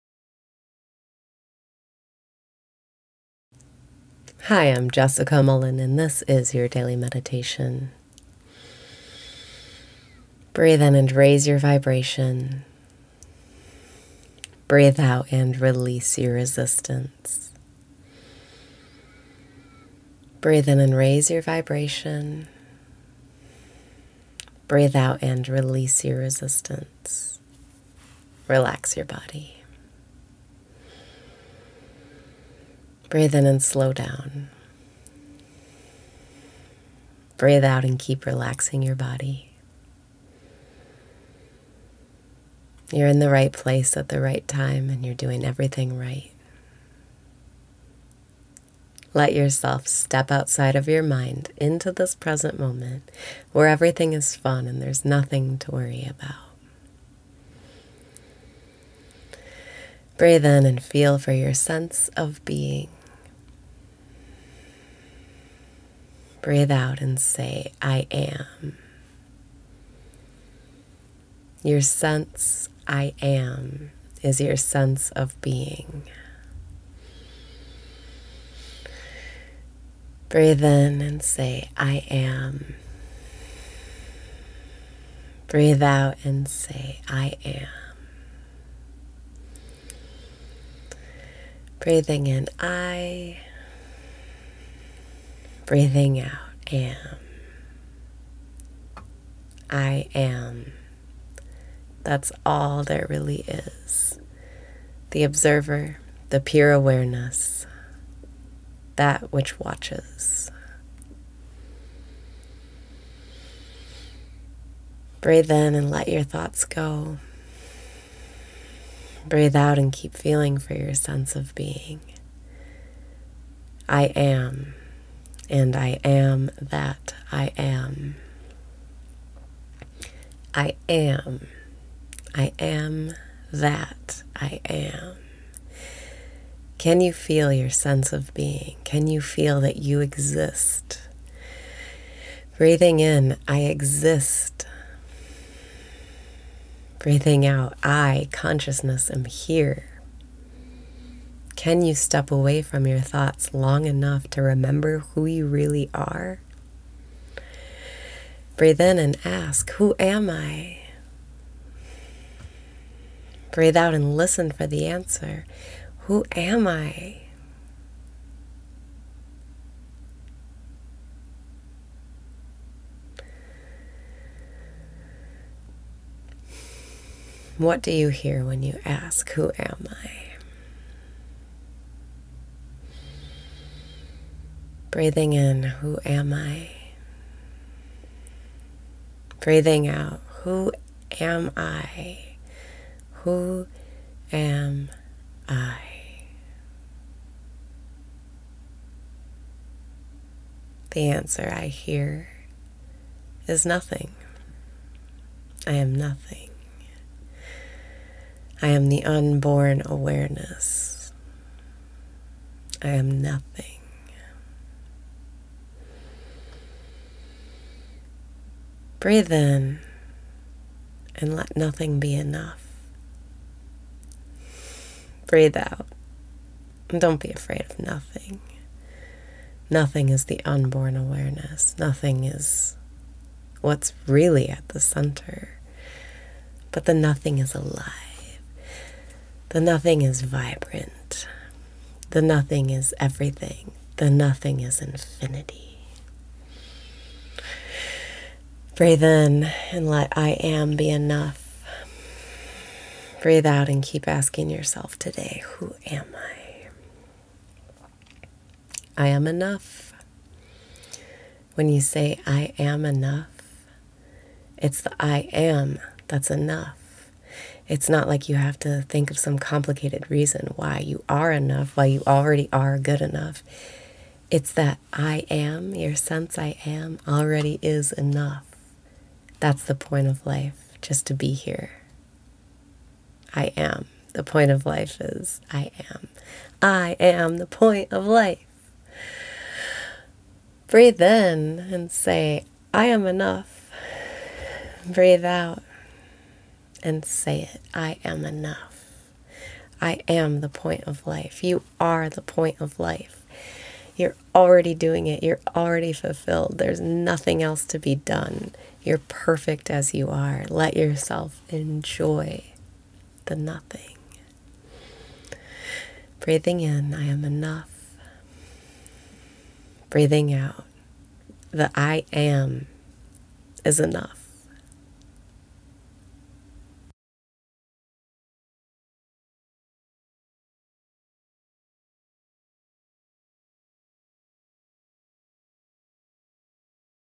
Daily Meditation - You Are the Point of Life
Breathe out and keep relaxing your body.

You’re in the right place at the right time, and you’re doing everything right. Let yourself step outside of your mind, into this present moment, where everything is fun and there’s nothing to worry about!

Breathe in and feel for your sense of being.
Breathe out and say, “I AM.”

Your sense I AM is your sense of being.

Breathe in and say, “I AM.”

Breathing out AM

I am. That’s all there really is. The observer, the pure awareness, that which watches.

Breathe in and let your thoughts go.
Breathe out and keep feeling for your sense of being.

I am. And I am that I am. I am. I am that I am. Can you feel your sense of being? Can you feel that you exist?

Breathing in I exist!
Breathing out I, consciousness, am here!

Can you step away from your thoughts long enough to remember who you really are?

Breathe in and ask, “Who am I?”
Breathe out and listen for the answer. Who am I?

What do you hear when you ask, Who am I?

Breathing in who am I?
Breathing out who am I?

Who am I? The answer I hear is, nothing. I am nothing. I am the unborn awareness. I am nothing.

Breathe in and let nothing be enough.
Breathe out and don’t be afraid of nothing!

Nothing is the unborn awareness. Nothing is what’s really at the center. But the nothing is alive! The nothing is vibrant! The nothing is everything! The nothing is infinity!

Breathe in and let I AM be enough!
Breathe out and keep asking yourself today, “Who am I?”

I am enough! When you say I AM enough, it’s the I AM that’s enough! It’s not like you have to think of some complicated reason why you are enough—why you already are good enough. It’s that I am—your sense I am—already is enough! That’s the point of life! Just to be here! I am the point of life. The point of life is I AM. I AM the point of life!

Breathe in and say I AM ENOUGH!
Breathe out and say it: I am enough.

I am the point of life. You are the point of life. You’re already doing it. You’re already fulfilled. There’s nothing else to be done! You’re perfect as you are. Let yourself enjoy the nothing!

Breathing in I AM ENOUGH!
Breathing out, the I AM is enough!